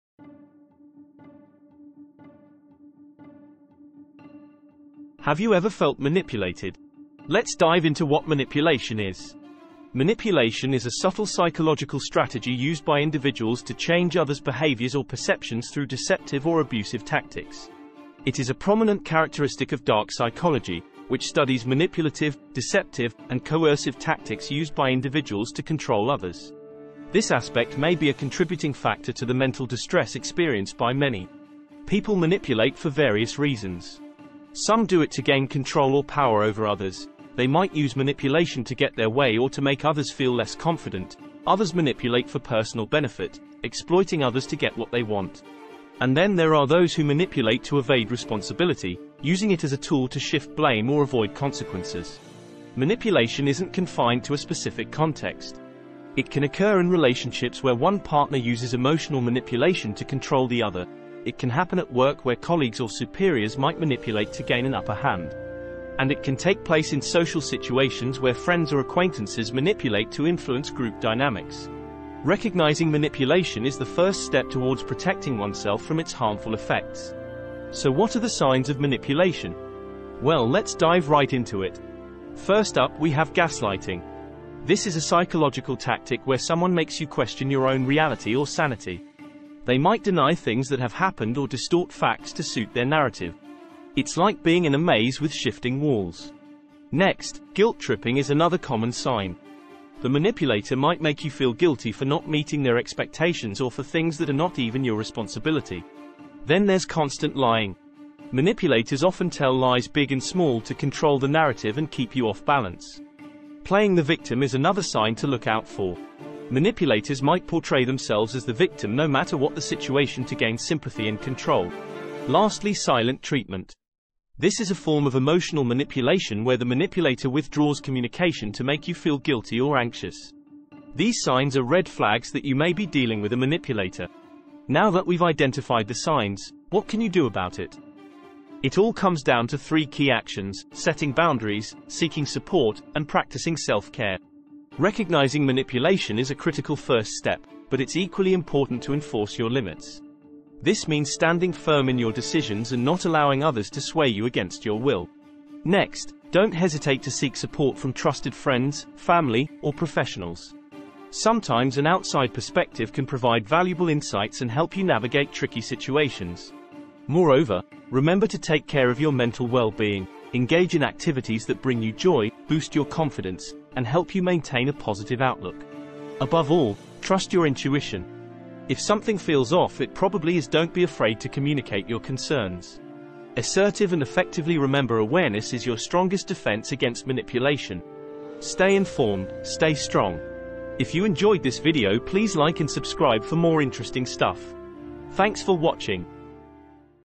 Manipulation and Dark Psychology
Have you ever felt manipulated? In this video, we explore the subtle psychological strategies individuals use to manipulate others and how it can impact our mental and emotional well-being. It is a prominent characteristic of Dark Psychology, which studies manipulative, deceptive, and coercive tactics used by individuals to control others. This aspect may be a contributing factor to the mental distress experienced by many. Manipulation takes various forms, from gaining control to exploiting others for personal benefit. Whether it's in relationships, the workplace, or social settings, recognizing the signs is crucial for self-protection.

We delve into common signs of manipulation, including gaslighting, guilt-tripping, constant lying, playing the victim, and the silent treatment. These red flags can help you identify manipulative behavior in your interactions. Once recognized, taking action is key.

Learn three essential strategies to deal with manipulation: set boundaries, seek support from trusted individuals, and prioritize self-care. Enforcing boundaries is vital in standing firm against manipulation, while seeking support provides valuable insights from external perspectives. Finally, practicing self-care ensures you maintain mental well-being and a positive outlook.

Remember, awareness is your strongest defense against manipulation. Stay informed, stay strong, and trust your intuition. Assertively address concerns to maintain a healthy and balanced life.